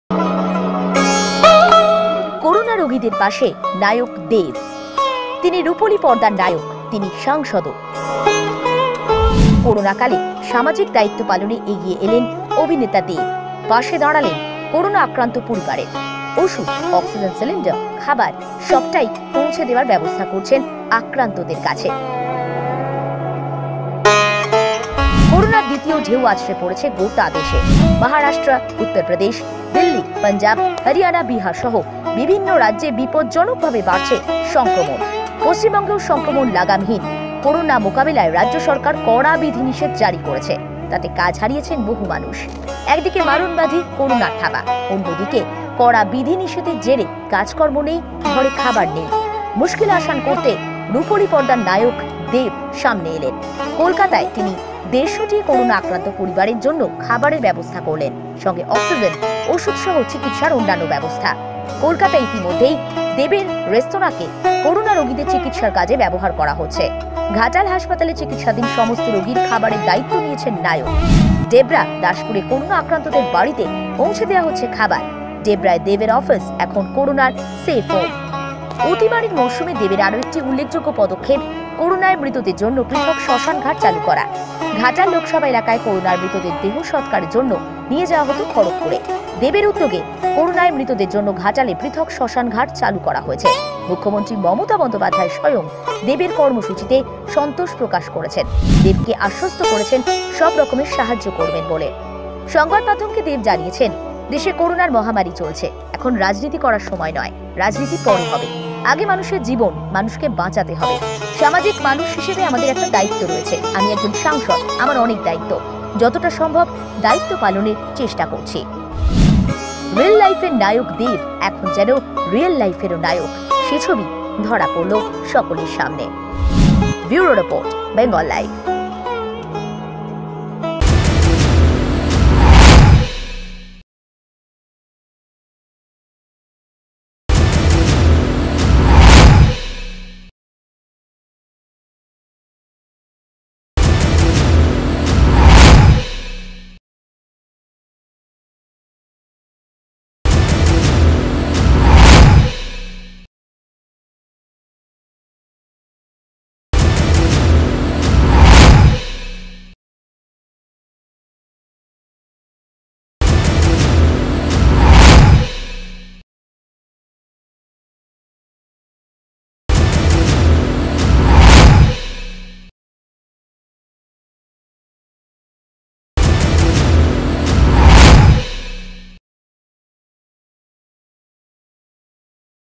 Hero "Dev" next to corona patients | করোনা রোগীদের পাশে নায়ক দেব
Bengal Live: Hero "Dev" next to corona patients.
করোনা রোগীদের পাশে নায়ক দেব.
তিনি রুপোলি পর্দার নায়ক। ‌তিনি সাংসদও। করোনাকালে সামাজিক দায়িত্ব পালনে এগিয়ে এলেন অভিনেতা দেব। পাশে দাঁড়ালেন করোনা আক্রান্ত পরিবারের। ওষুধ, অক্সিজেন সিলিন্ডার থেকে আক্রান্তদের খাবার--তিনি সব কিছুরই ব্যবস্থা করেছেন।

করোনার দ্বিতীয় ঢেউ আছড়ে পড়েছে গোটা দেশে। ‌ মহারাষ্ট্র, উত্তরপ্রদেশ, দিল্লি, পাঞ্জাব, হরিয়ানা ও বিহার-সহ দেশের বিভিন্ন রাজ্যে বিপজ্জনকভাবে বাড়ছে সংক্রমণ। পশ্চিমবঙ্গেও সংক্রমণ লাগামহীন। করোনা মোকাবিলায় রাজ্য সরকার লকডাউন জারি করেছে। তাতে কাজ হারিয়েছেন বহু মানুষ। একদিকে মারণব্যাধি করোনার থাবা। অন্যদিকে লকডাউনে ঘরে খাবার নেই। মুশকিল আসান রুপোলি পর্দার নায়ক দেব। কলকাতায় তিনি ১৫০টি করোনা আক্রান্ত পরিবারকে খাবারের ব্যবস্থা করেছেন। সঙ্গে অক্সিজেন, ওষুধ-সহ চিকিৎসার অন্যান্য ব্যবস্থা। কলকাতায় দেবের রেস্তোরাঁকে করোনা রোগীদের চিকিৎসার কাজে ব্যবহার করা হচ্ছে। ঘাটাল হাসপাতালে চিকিৎসাধীন সমস্ত রোগীর খাবারের দায়িত্ব নিয়েছেন নায়ক। ডেবরা ও দাসপুরে করোনা আক্রান্তদের বাড়িতে পৌঁছে দেওয়া হচ্ছে খাবার। ডেবরায় দেবের অফিস এখন করোনার সেভ হোম।

অতিমারির মরশুমে দেবের আরও একটি উল্লেখযোগ্য পদক্ষেপ হল করোনায় মৃতদের জন্য পৃথক শ্মশানঘাট চালু করা। ঘাটাল লোকসভা এলাকায় করোনায় মৃতদের দেহ সৎকারের জন্য নিয়ে যাওয়া হত খড়্গপুরে। ‌ দেবের উদ্যোগে করোনায় মৃতদের জন্য ঘাটালে পৃথক শ্মশানঘাট চালু করা হয়েছে। ‌মুখ্যমন্ত্রী মমতা বন্দ্যোপাধ্যায় দেবের কর্মসূচিতে সন্তোষ প্রকাশ করেছেন। দেবকে তিনি আশ্বস্ত করেছেন সবরকম সাহায্য করবেন বলে।

সংবাদমাধ্যমকে দেব জানিয়েছেন, 'দেশে করোনার মহামারী চলছে। এখন রাজনীতি করার সময় নয়। রাজনীতি পরে করা যাবে। আগে মানুষের জীবন। মানুষকে বাঁচাতে হবে। সামাজিক মানুষ হিসেবে আমাদের প্রত্যেকের একটা দায়িত্ব আছে। আমি একজন সাংসদ। আমার অনেক দায়িত্ব। তাই যতটা সম্ভব আমি সেই দায়িত্ব পালনে চেষ্টা করছি।'

রিল লাইফের নায়ক দেব যেন‌ রিয়েল লাইফেও নায়ক।